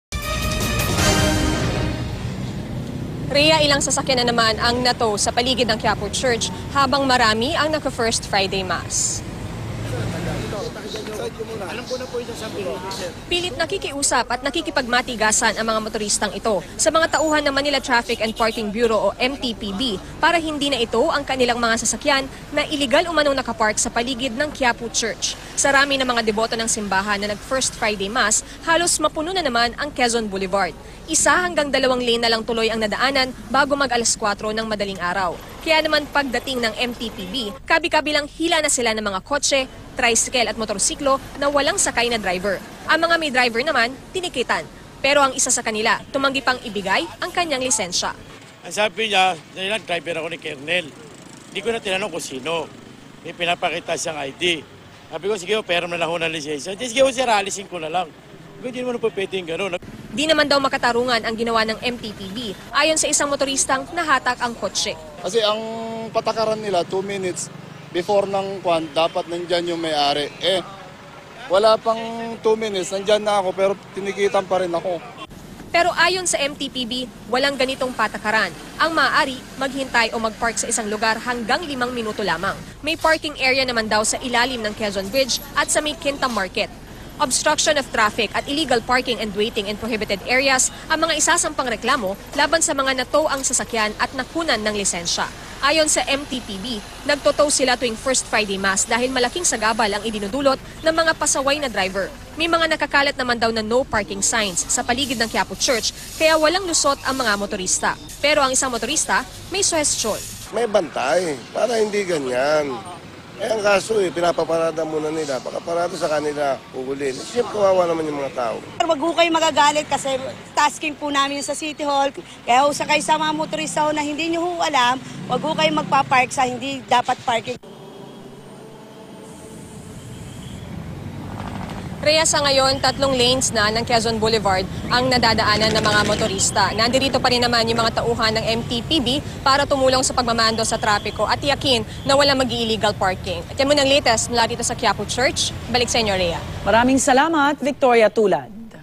UB: Mga sasakyang ilegal na nakaparada sa paligid ng Quiapo Church, hinatak
Umalma ang mga may-ari ng ilang sasakyang hinatak ng Manila Traffic and Parking Bureau dahil ilegal na nakaparada sa paligid ng Quiapo Church. Narito ang unang balita ni Victoria Tulad.